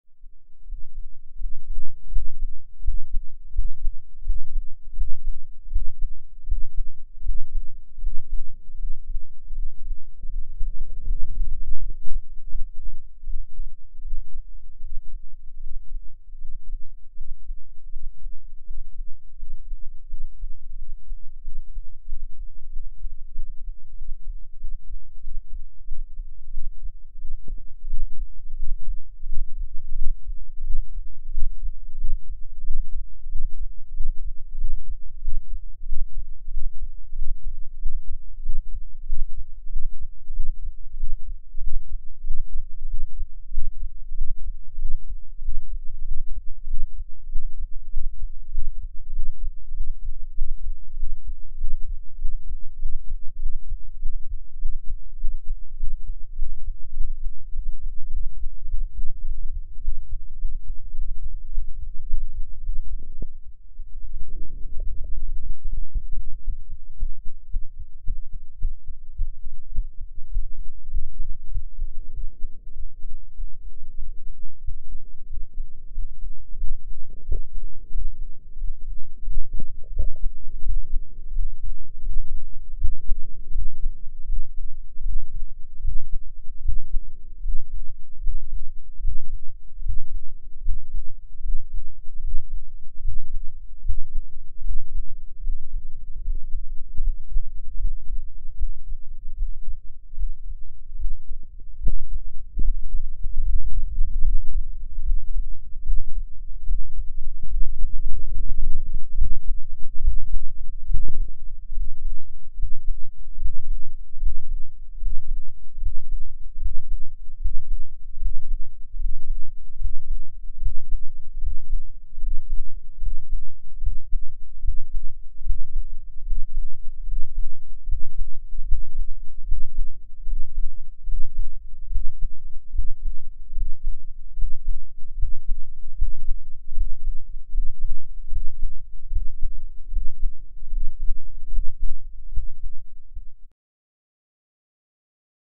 Heartbeat: Testing another microphone enclosure
Experimenting with different microphones and tubes. This one turned out, in a sense, too good. Dominating frequency is about 10 Hz, so my heartheat can be heard only in decent headphones, as low thumping. Those without good headphones, sorry, I will make something for you next time 😉. The record also contains a breathhold, although breathholds do very little to the rhythm of my heart...